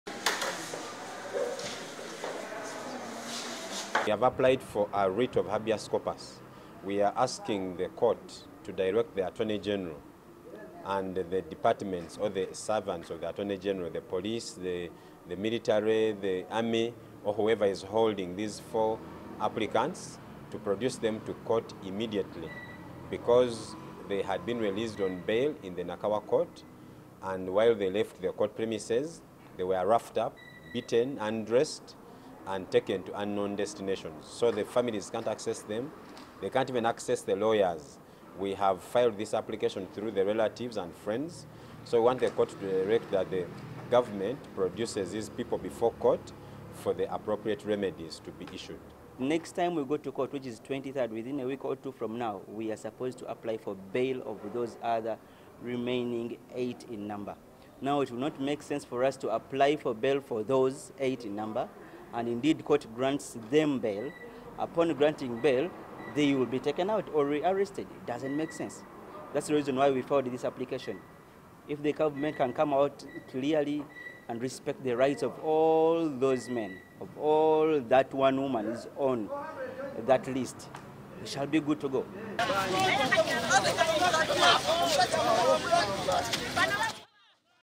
Kaweesi Murder Suspect's lawyers want their clients released by security agencies
Lawyers of the suspects charged with the murder of three people including former police spokesperson Andrew Felix Kaweesi have gone to court to seek justice for their clients. They contend that the men’s personal liberty was infringed upon when they were re-arrested, minutes after they had been released on bail on Tuesday, and their whereabouts are still unknown. The advocates want court to order the Attorney General to produce their clients in court. Seven suspects were granted bail by Nakawa magistrate Noah Sajjabi.